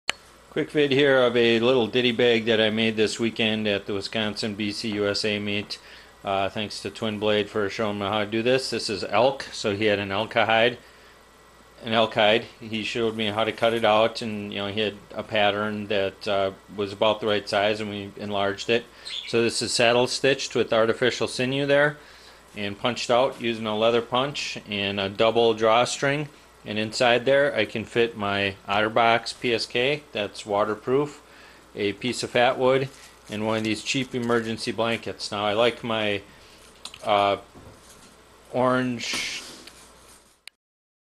Hand made Elk Hide ditty bag sewn with artificial sinew
This was a very cool project to do.  Everybody should learn how to use the saddle stitch.